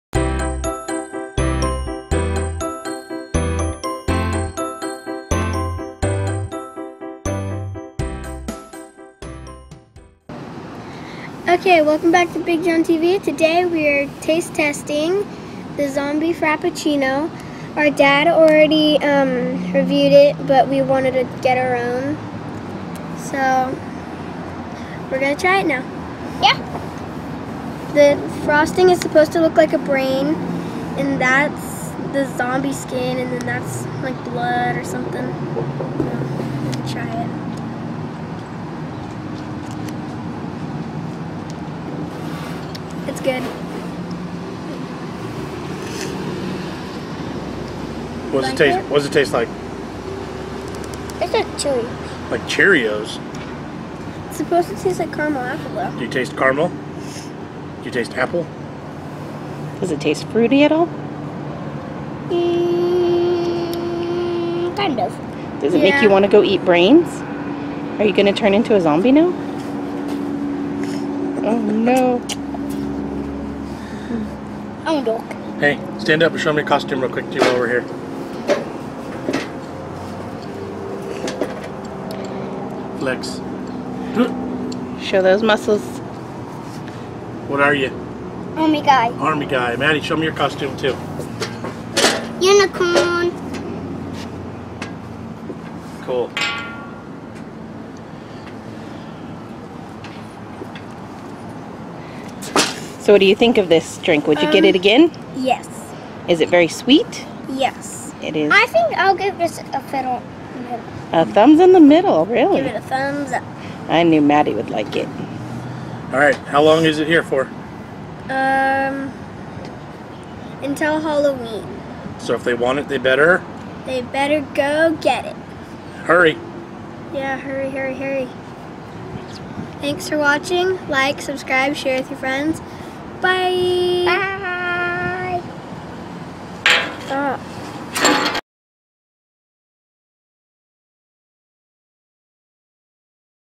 Kids Review || Zombie Frappuccino
The kids were bummed they didn't get to try the Limited Edition Zombie Frappuccino from Starbucks, so we let them review their own!